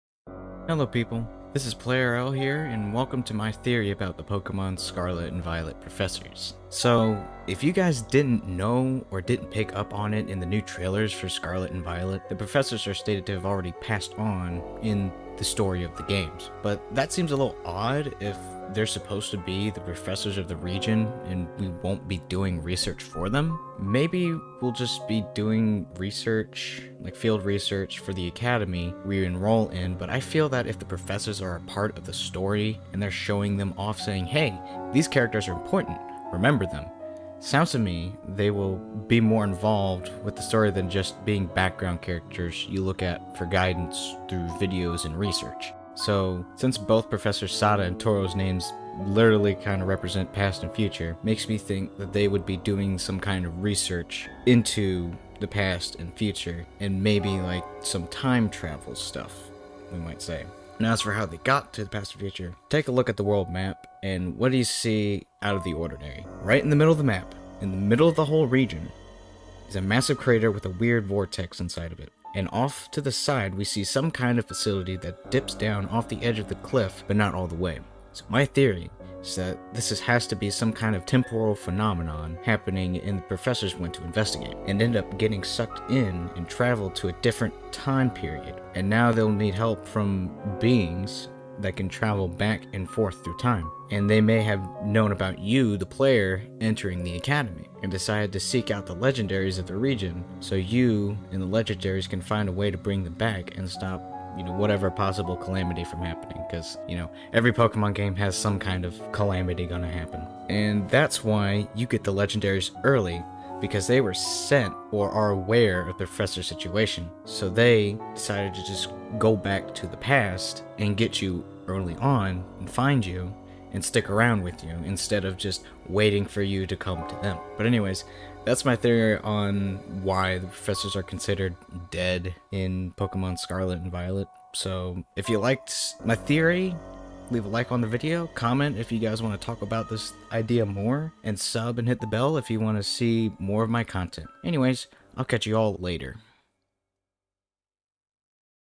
The Case of the Pokemon Scarlet and Violet Professors || Pokemon Scarlet and Violet Theory
Hello, welcome to my Theory about what really happened to the Pokemon Scarlet and Violet Professors.